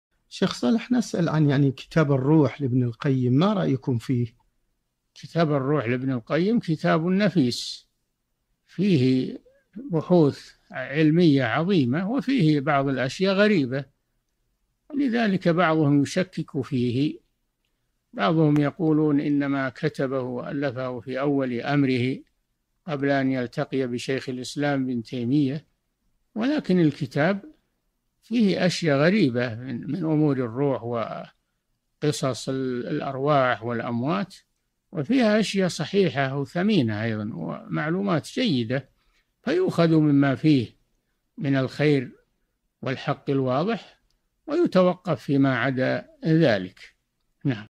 05 من 06|أسئلة شرح كتاب التوحيد|كتب التوحيد|ما رأيكم في كتاب الروح لابن القيم؟|صالح الفوزان
السيرة الذاتية :
للاطلاع على أسئلة شرح كتاب التوحيد (كتب التوحيد) للشيخ صالح الفوزان
وللاطلاع على جميع حلقات شرح كتاب التوحيد (أصل المادة):
للاشتراك في قناة الشيخ صالح الفوزان